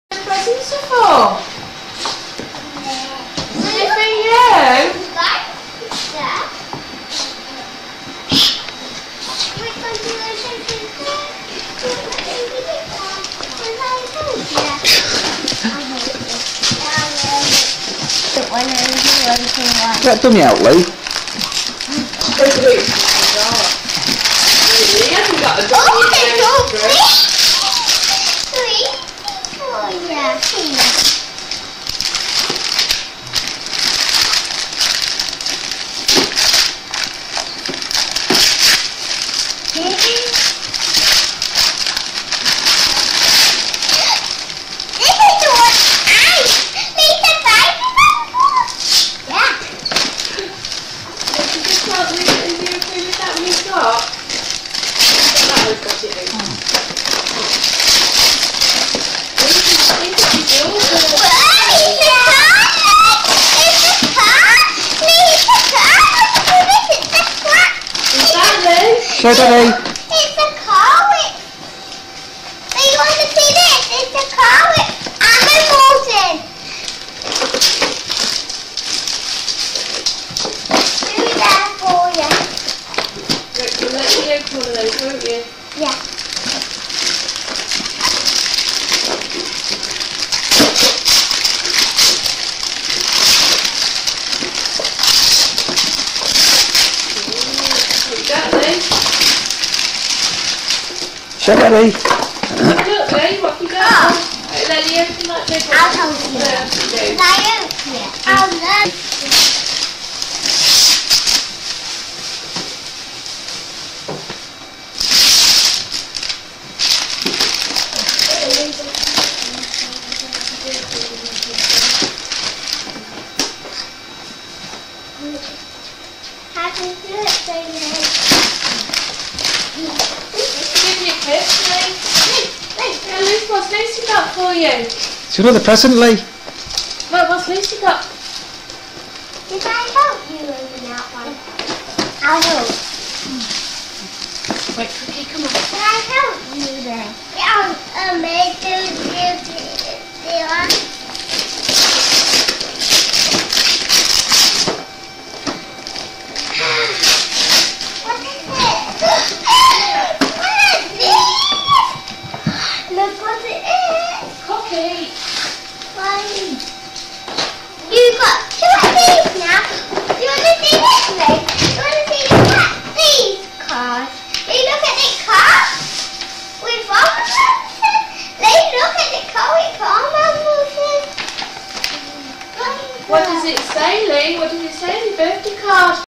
2004 Lee 2nd birthday
Lee now fully walking and learning to talk .. its his Second birthday!